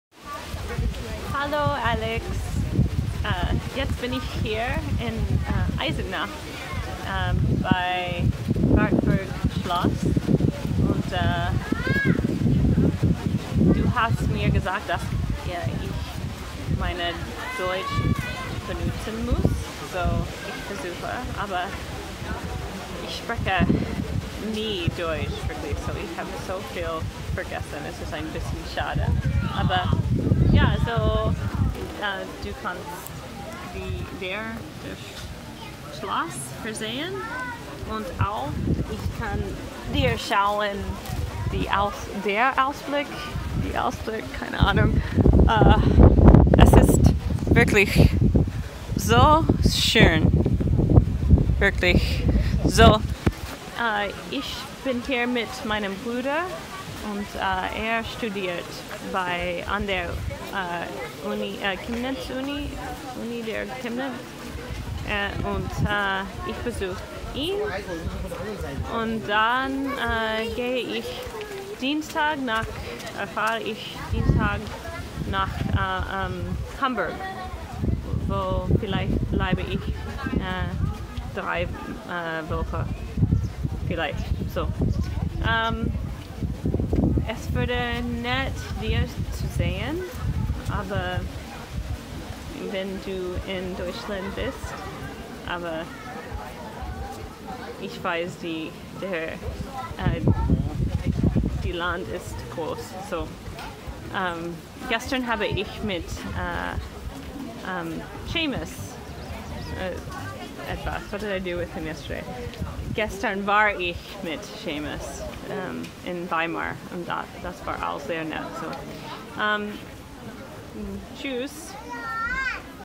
Me speaking German poorly while In Germany
Made at the Wartburg Castle in Germany-- in which I attempt to speak German